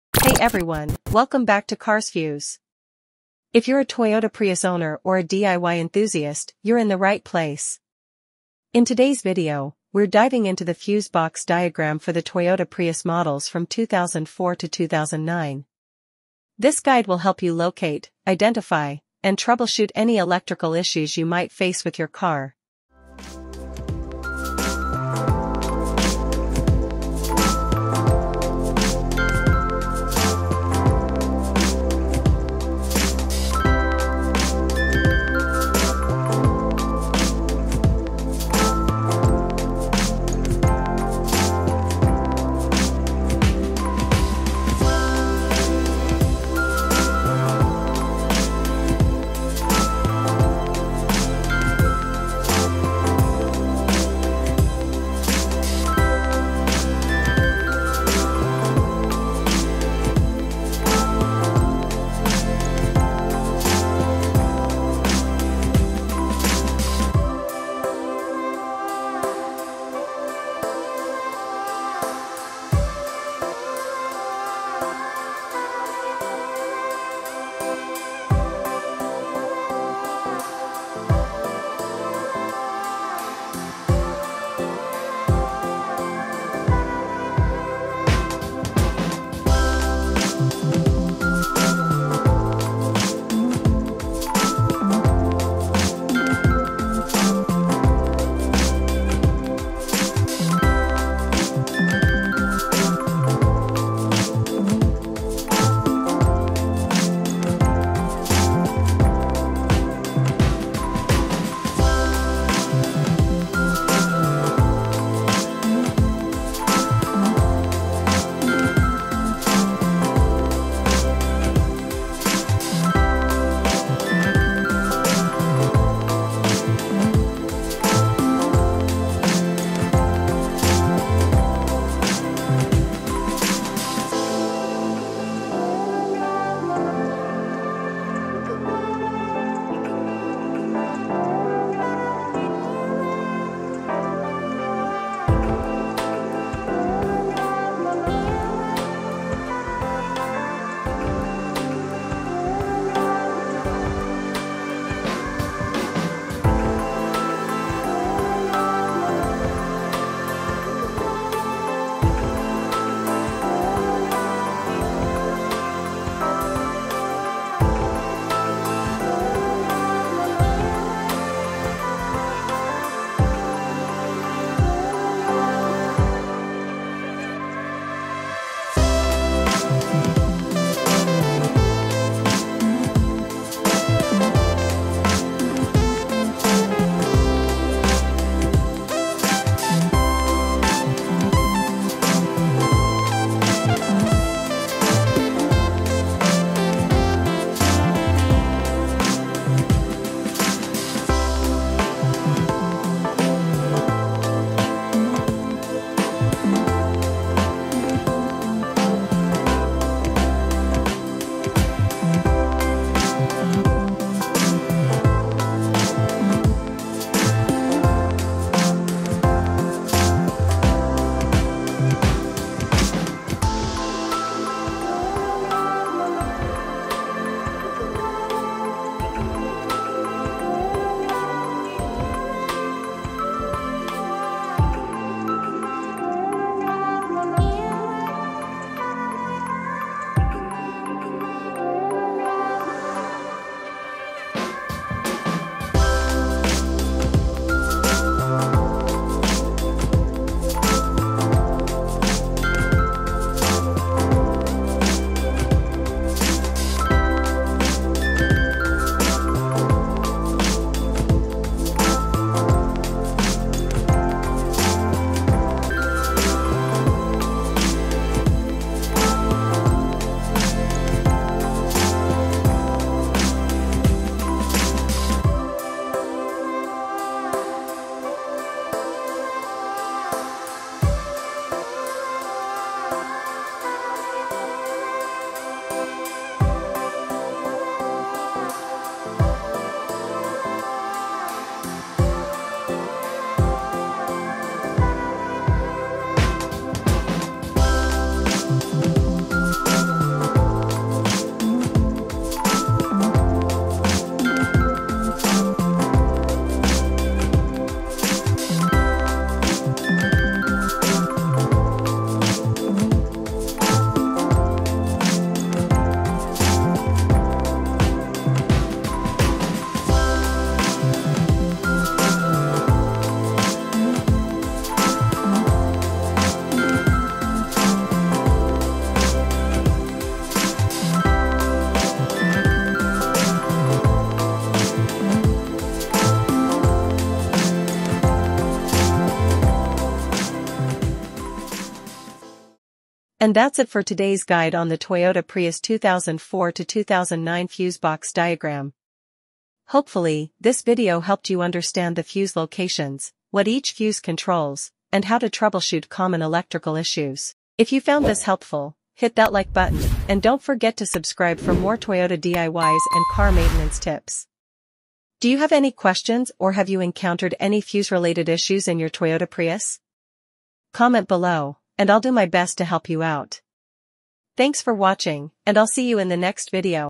Fusebox Location & Diagrams: (2004/2005/2006/2007/2008/2009) Toyota Prius (XW20)
The second-generation Toyota Prius (XW20), made from 2003 to 2009.

You can find details about where the fuse panels are located inside the car, along with information on each fuse’s purpose and relay assignments for model years 2004 to 2009.